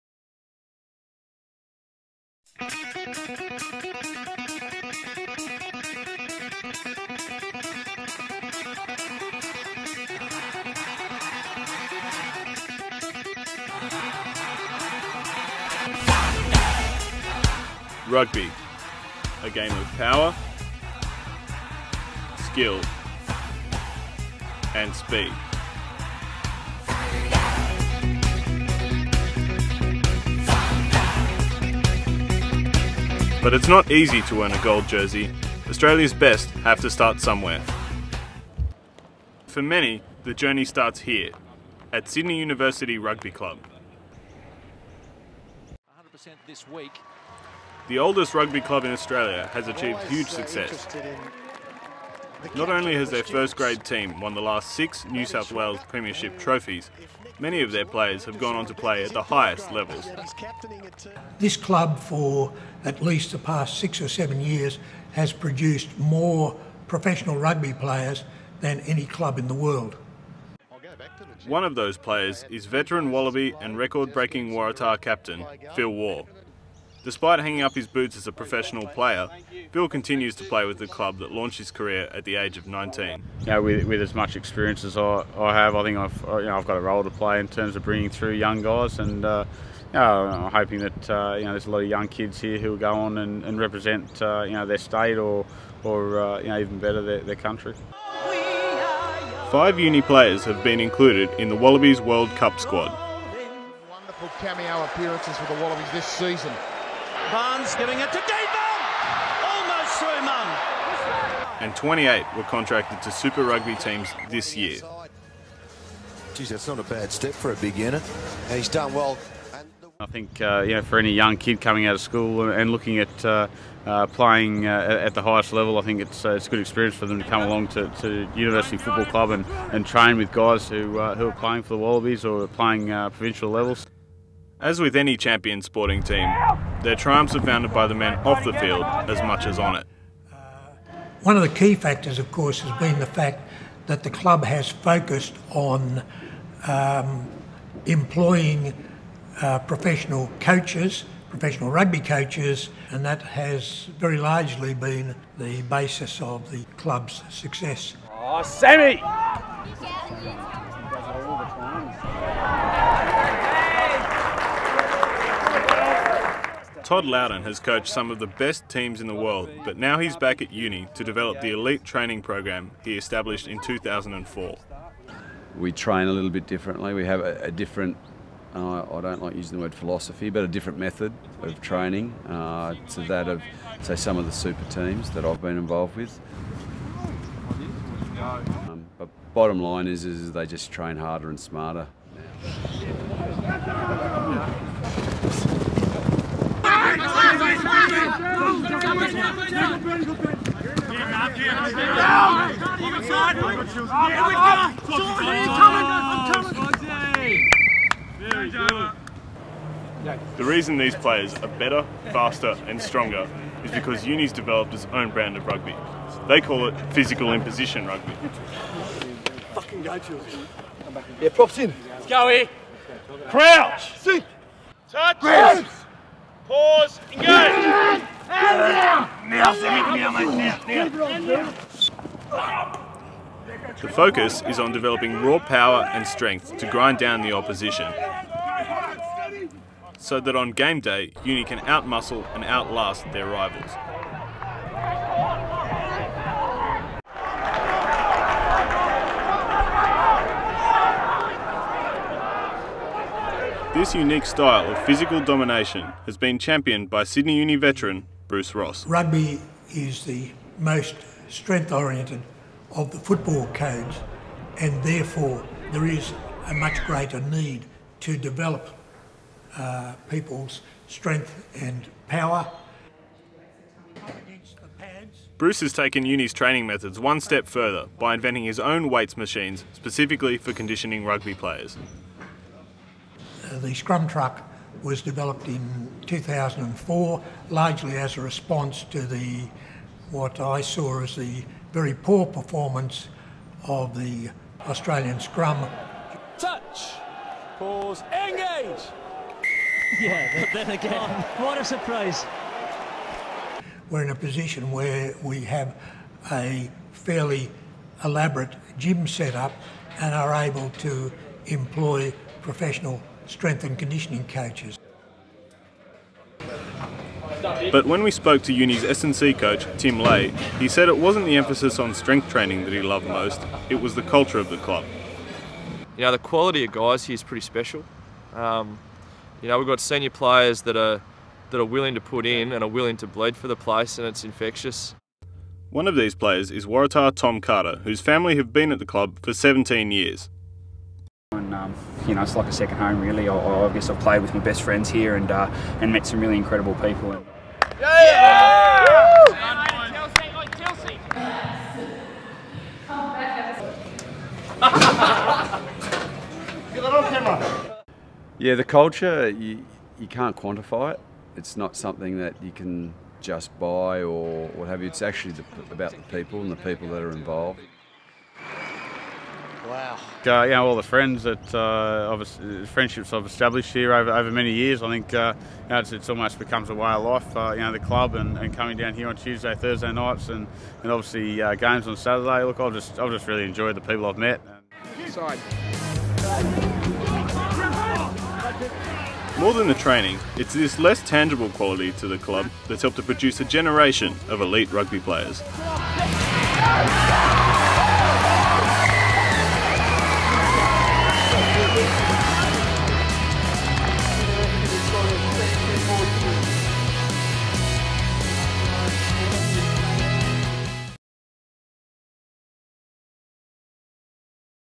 Sandstone Rugby Factory
Why has Sydney University's rugby club produced more elite players than any other over the last decade?

Jack Norton (aka Jnor), Nick Jordan and Vivien Luu